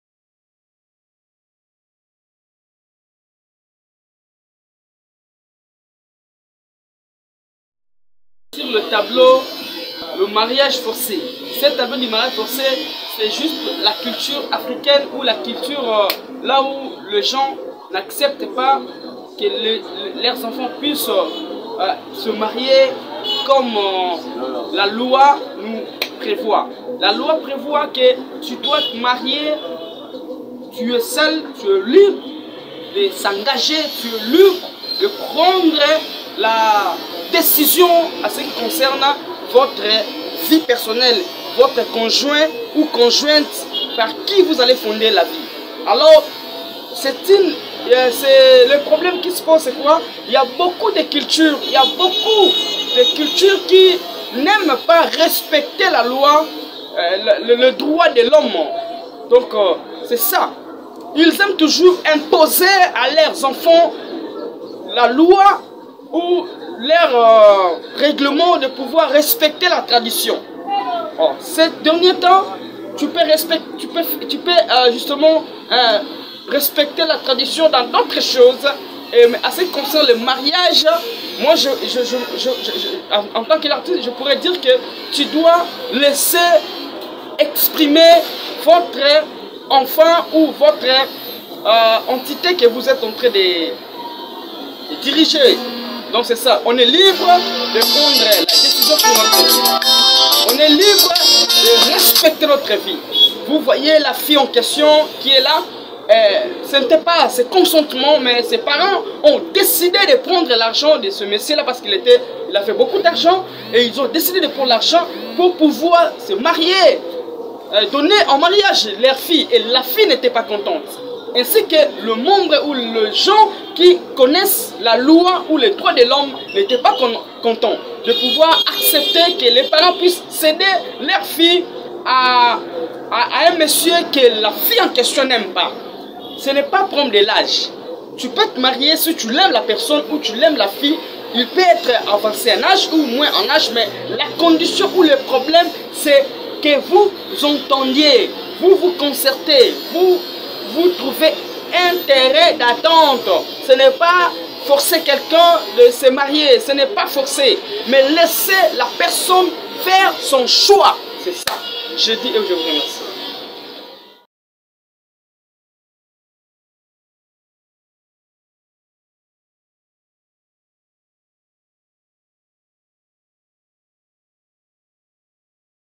JC Lofenia Explique sa toile Contemporary African Art Peinture Populaire Congolaise
Le Mariage forcé réalisée en 2018